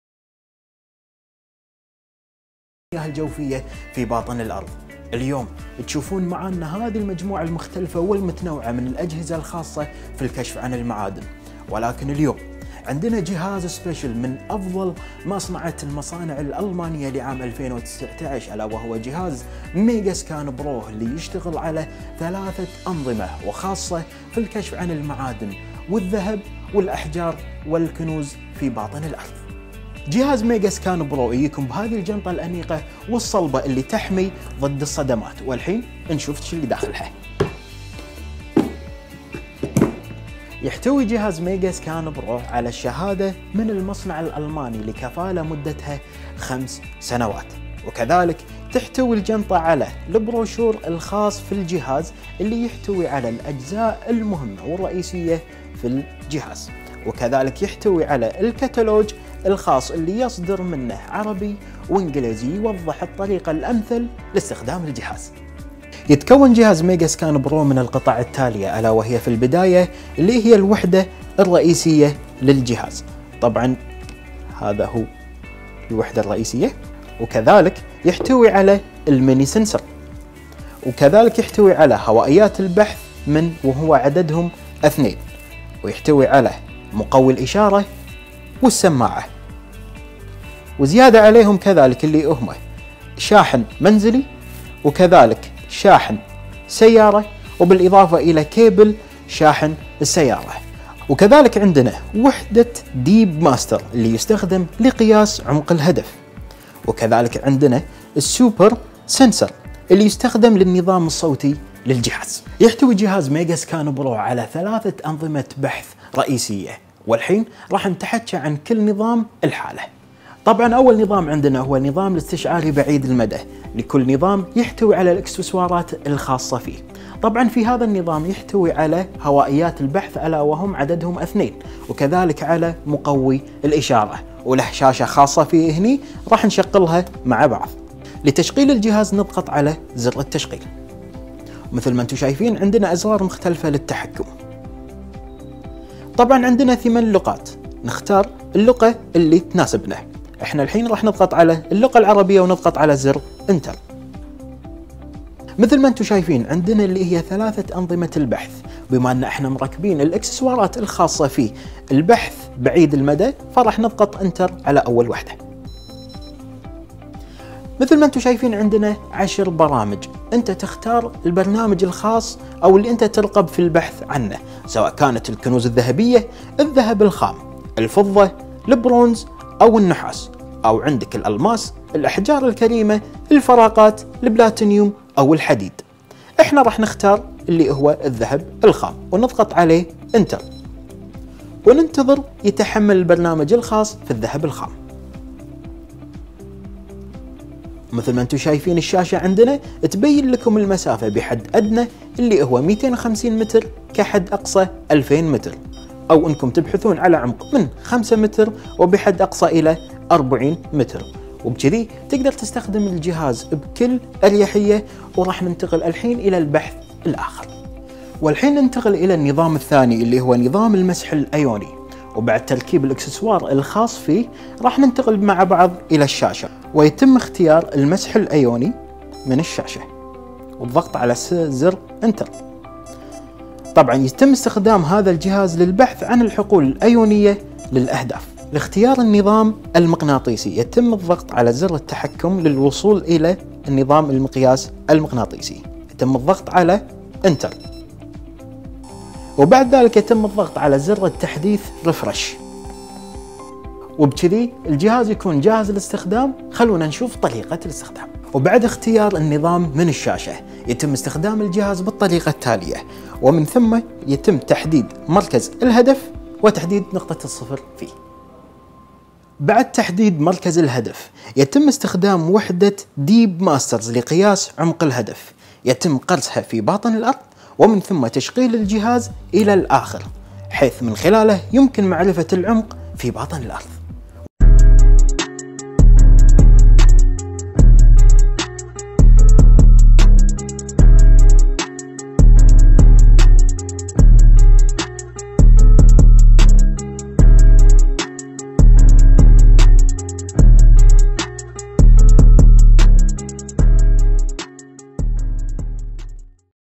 اجهزة كشف الذهب فى الامارات | جهاز كشف الذهب ميجا سكان برو - افضل سعر فى دبي
جهاز كشف الذهب فى الامارات | ميجا سكان برو - MEGA SCAN PRO | أفضل اجهزة كشف الذهب والمعادن

شركة جولد ماستر تقدم افضل سعر جهاز كشف الذهب فى دبي
كاشف الذهب والكنوز الاحدث عالميا ميجا سكان برو 2019  المتطور

يعمل بثلاث انظمه بحث
النظام الاستشعاري بعيد المدي
النظام الايوني
النظام المغناطيسي

لطلب الجهاز يرجي  التواصل مع قسم المبيعات

العنوان : الامارات - دبي - ديرة - شارع المكتوم - رقة البطين بلازا - الطابق الخامس 506